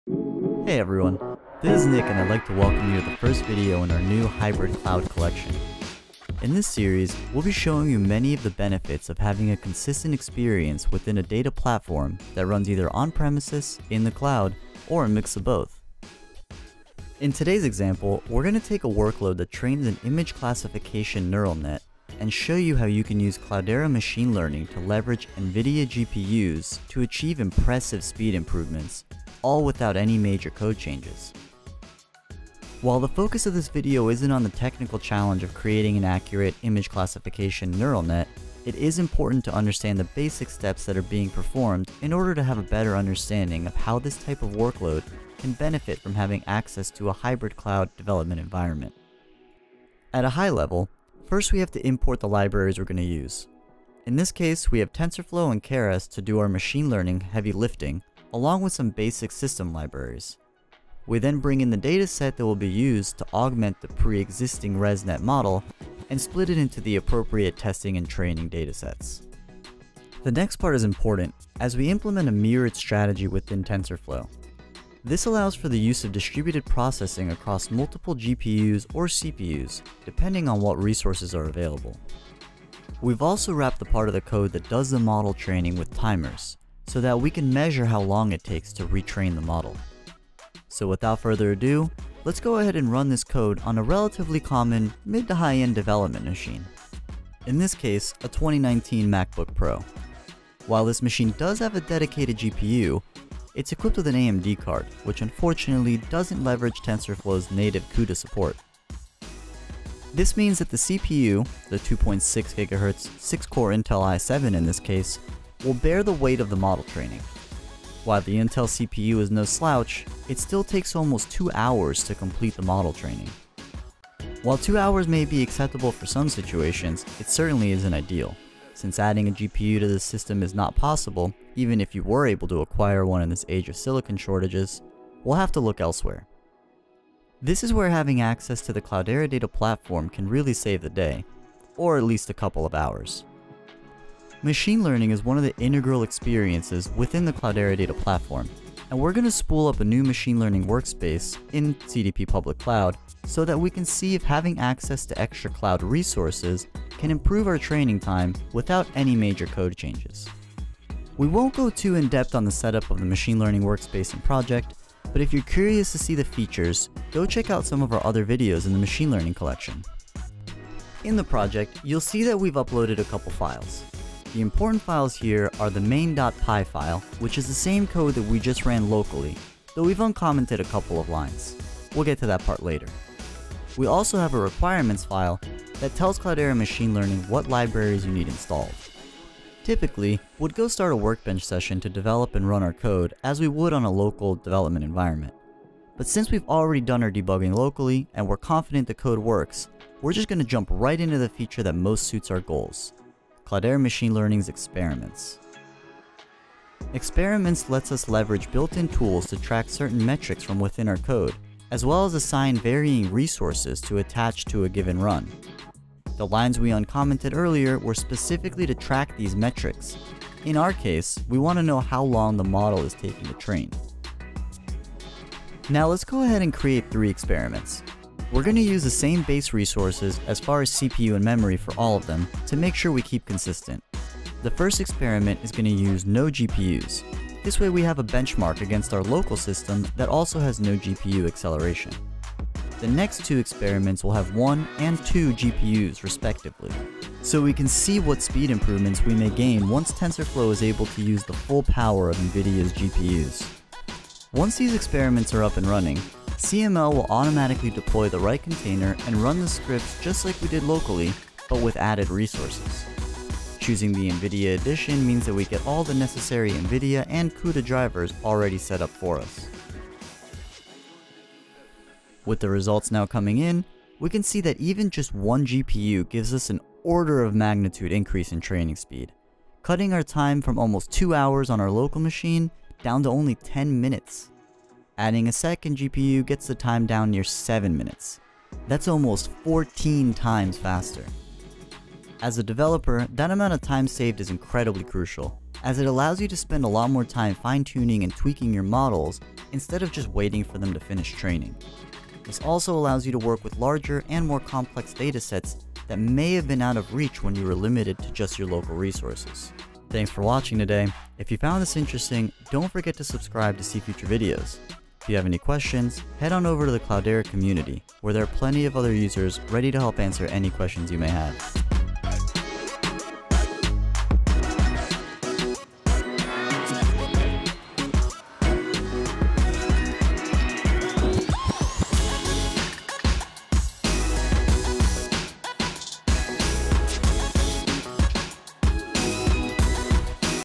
Accelerating AI Training Using GPUs on Cloudera Machine Learning
In today’s video, we’re going to take a workload that trains an image classification neural net and show how you can use Cloudera Machine Learning to leverage Nvidia GPUs to achieve impressive speed improvements without any substantial code changes.